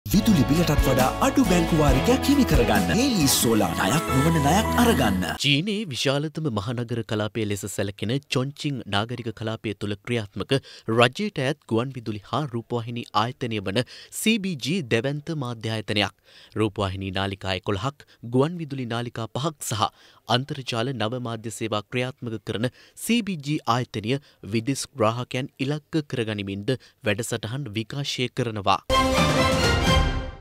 ජාතික රූපවාහිනිය සමග අත්වැල් බැඳගත් චීනයේ චොංචිං - CBG | Rupavahini News
ශ්‍රී ලංකා රූපවාහිනි සංස්ථාව සමග  පුළුල් මාධ්‍ය සහයෝගීතාවක් සඳහා චීනයේ "සී බී  ජී" හෙවත්, "චොංචිං" විකාශන සමූහය එකඟත්වය පළ කළා. රූපවාහිනී සංස්ථාවේ ඉහළ කළමනාකාරිත්වය සමග ඊයේ පැවති සාකච්ඡාවකදී එහි නියෝජිතයින් මේ බව කියා සිටියා.